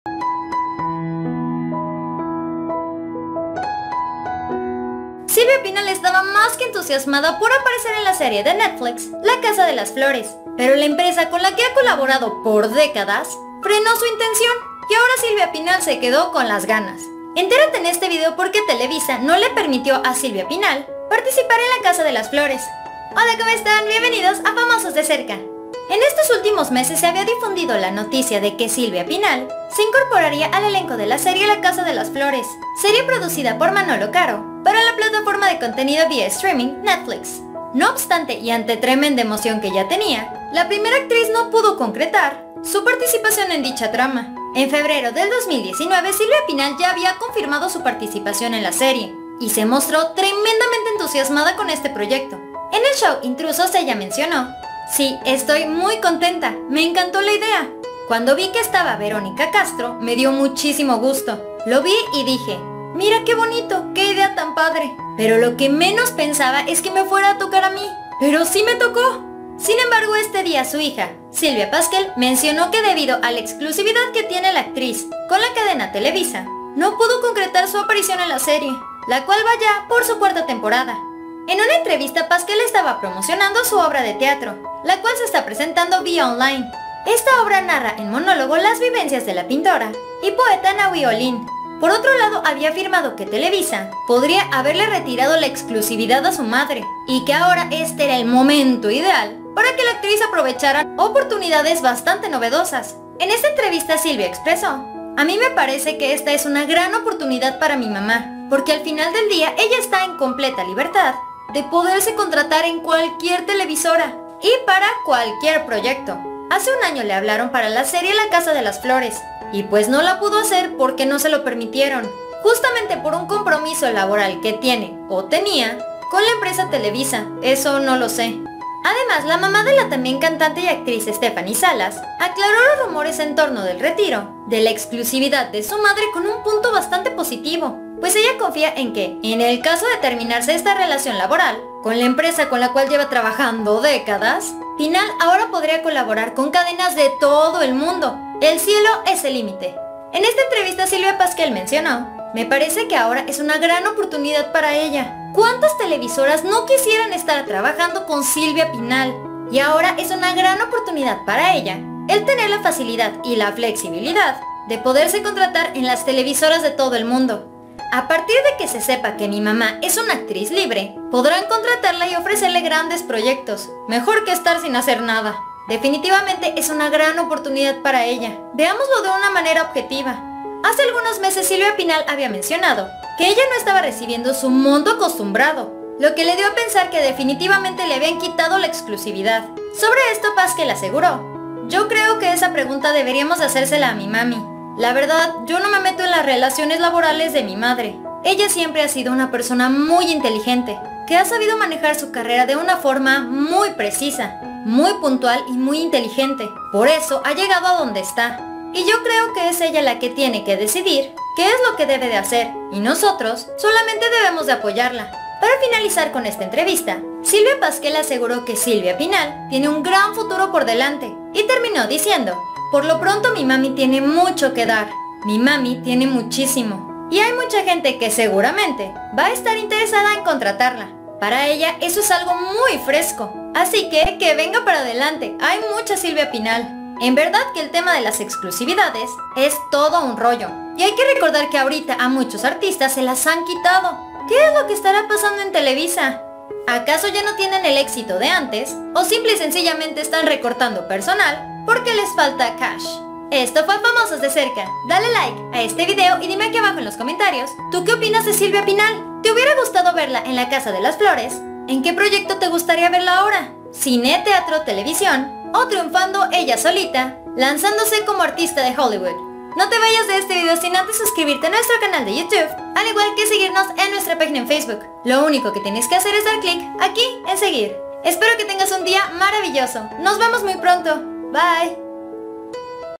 ⚠️ ¡ 𝗛𝗮𝗰𝗲 𝗨𝗻𝗼𝘀 𝗠𝗼𝗺𝗲𝗻𝘁𝗼𝘀 💔😭 𝗧𝗲𝗿𝗺𝗶𝗻𝗮𝗻 𝗚𝗥𝗔𝗡 𝗜𝗟𝗨𝗦𝗜Ó𝗡 de Silvia Pinal ! 🔕
Silvia Pinal Contra su voluntad es llevada a tomar esta triste decision.

 BIENVENIDOS!!!